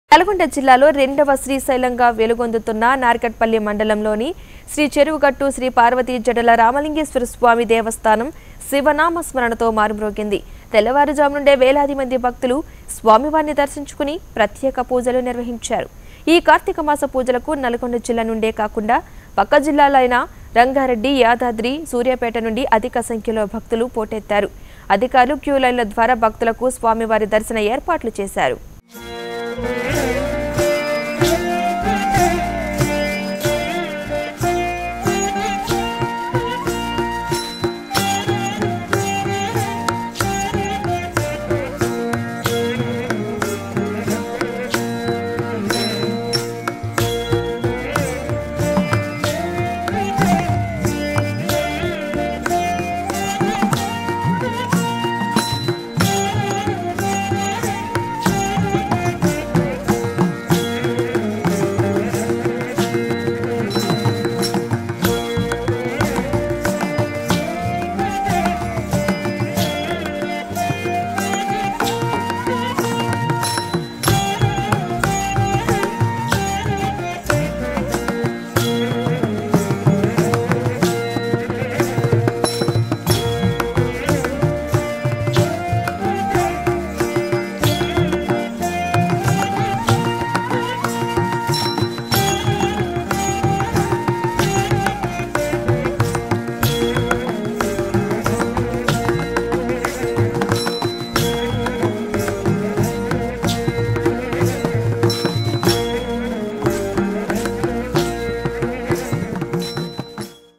నల్గొండ జిల్లా కార్తీక మాసం సంద‌ర్భంగా శ్రీ చెరువుగట్టు శ్రీ పార్వతి జడల రామలింగేశ్వర స్వామి దేవస్థా
Darshanam News (21-10-17) | నల్గొండ జిల్లా కార్తీక మాసం సంద‌ర్భంగా శ్రీ చెరువుగట్టు శ్రీ పార్వతి జడల రామలింగేశ్వర స్వామి దేవస్థా